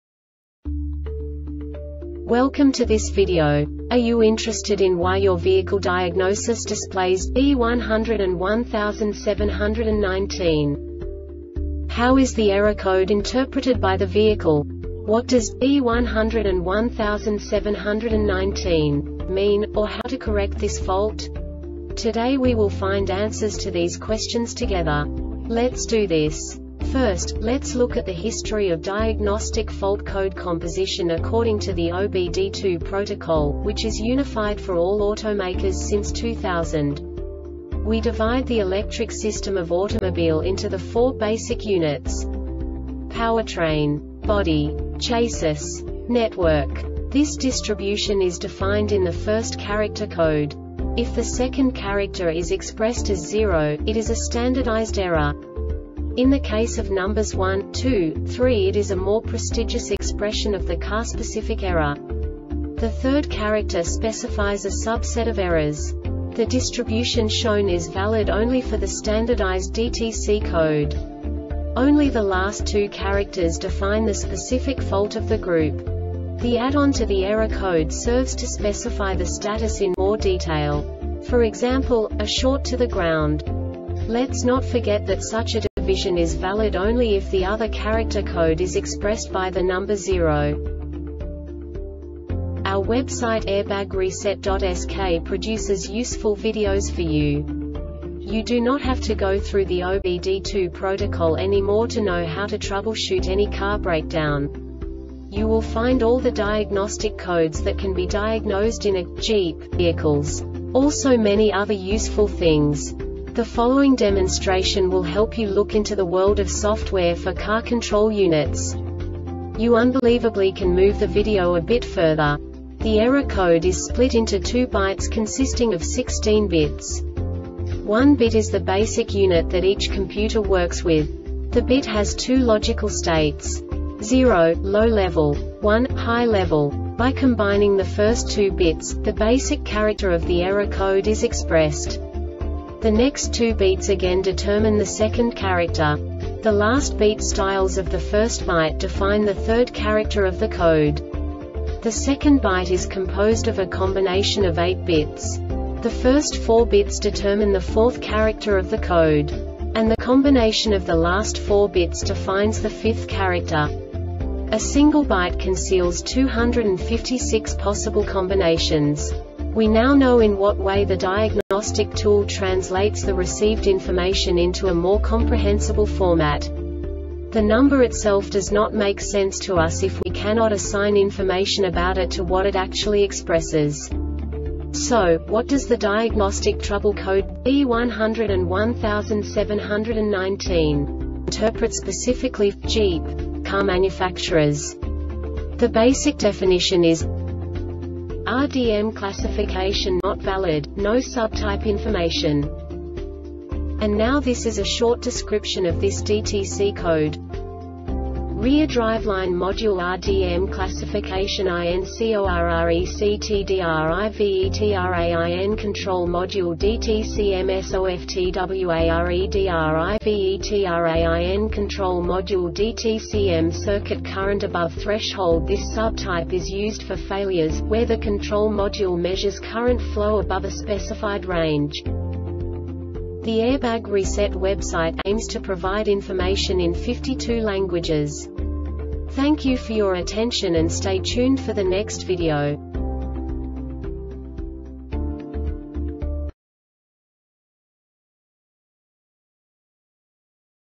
DTC Jeep B1017-19 Short Explanation
Contents:
0:21 Basic DTC analysis according to OBD2 protocol standard.
1:48 Insight into programming
2:58 A diagnostically understandable description of the Diagnostic Trouble Code
3:05 Basic error definition

Make:
Jeep
Code:
B1017 19
Definition:
RDM Classification Not Valid - No Subtype Information
Cause:
REAR DRIVELINE MODULE (RDM) CLASSIFICATION INCORRECTDRIVETRAIN CONTROL MODULE (DTCM) SOFTWAREDRIVETRAIN CONTROL MODULE (DTCM)
Failure Type: